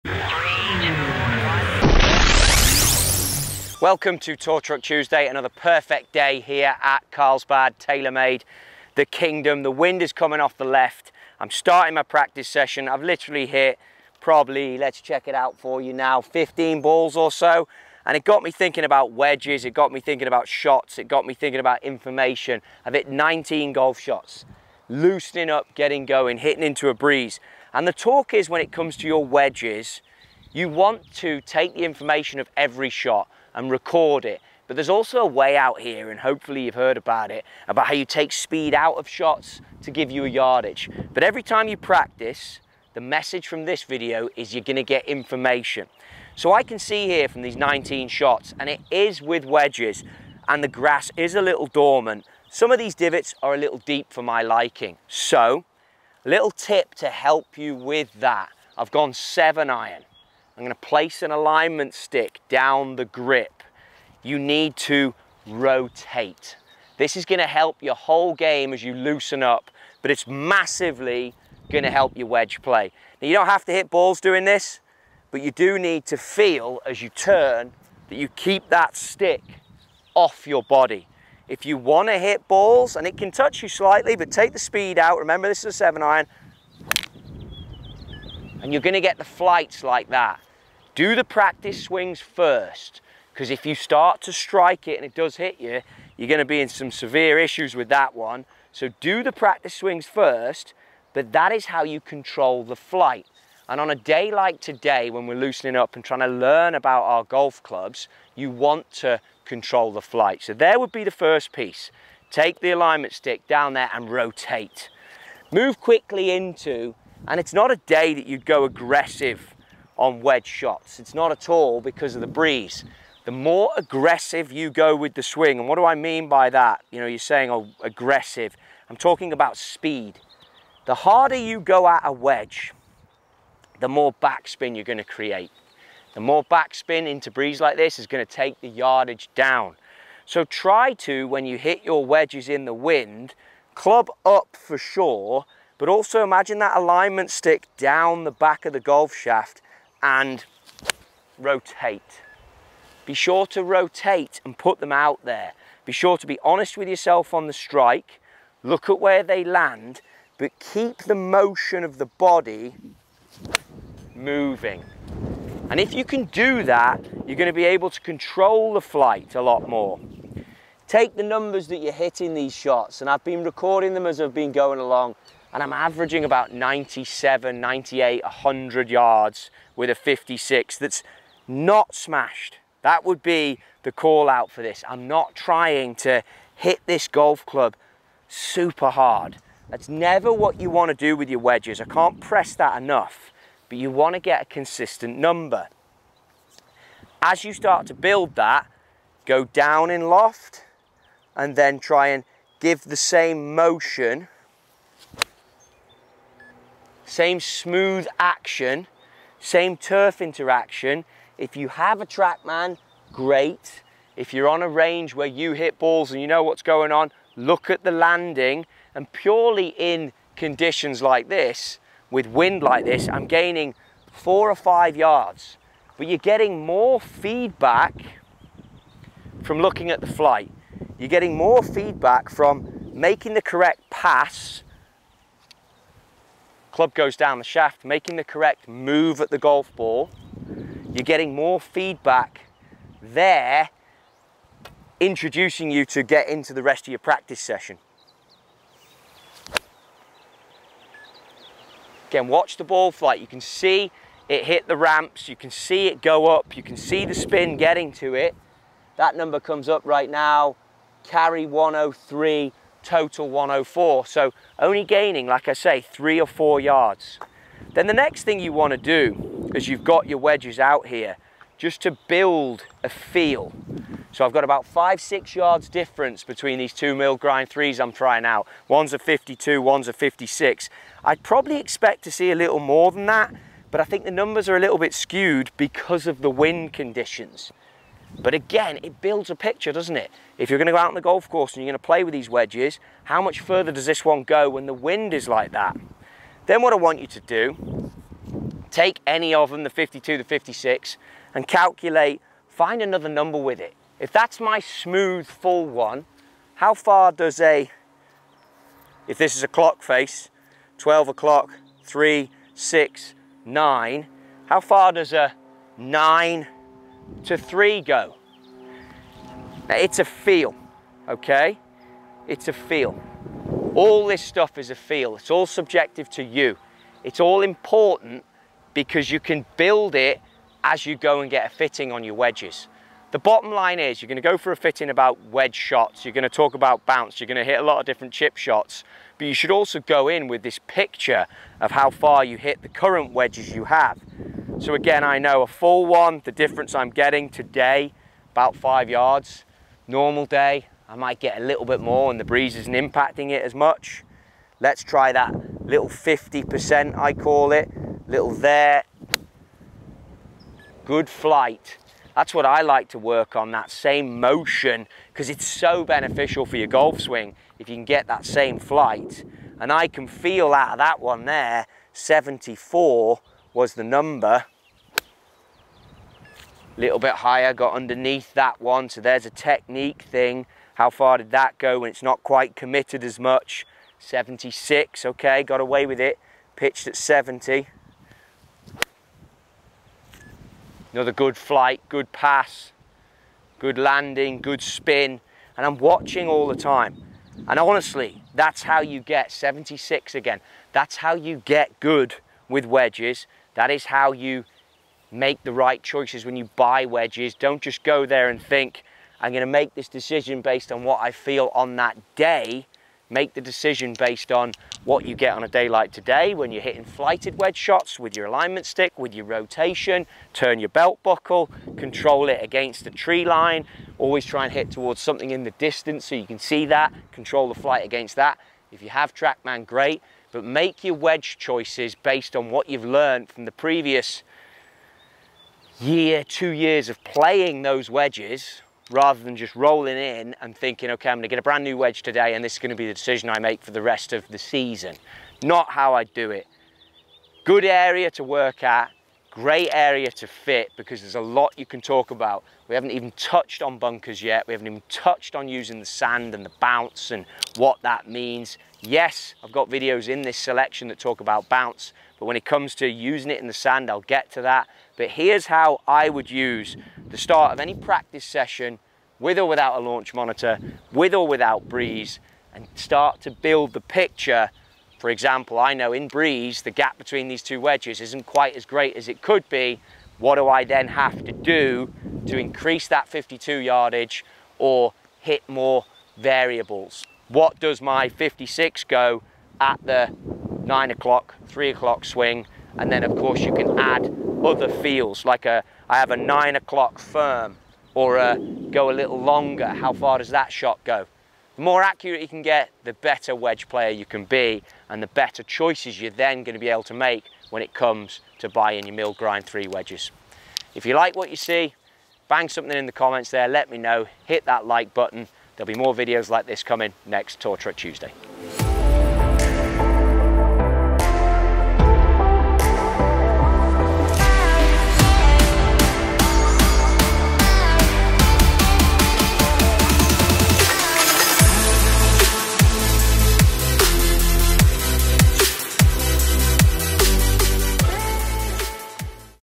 Understand Your Golf Wedges & HIT THE PERFECT FLIGHT | TrottieGolf
A wedge fitting doesn't have to just happen in your session with the FITR, you  can start to build the knowledge required to get the perfect wedge set up throughout your whole journey with your current clubs.

This video will help you identify yardage control, make you consider your practice routine and push you to consider turf interaction & learn HOW TO HIT THE PERFECT FLIGHTED WEDGE!